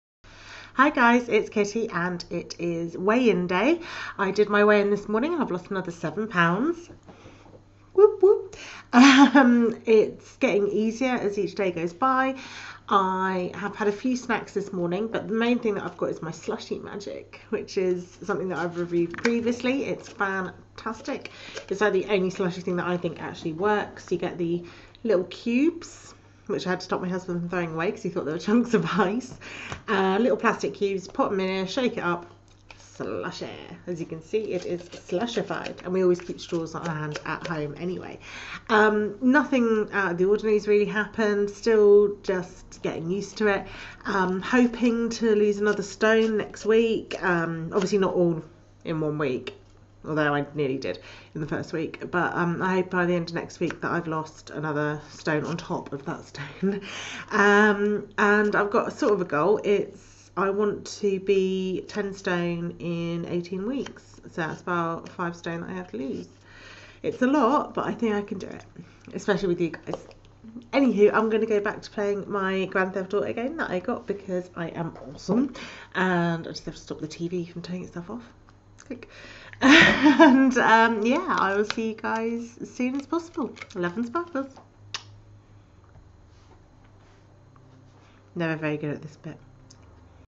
exante week two weigh in
Week two weigh in and another 7lbs GONE :) I really can't express just how AWESOME this diet is!

Love and Sparkles

Kitty
xxx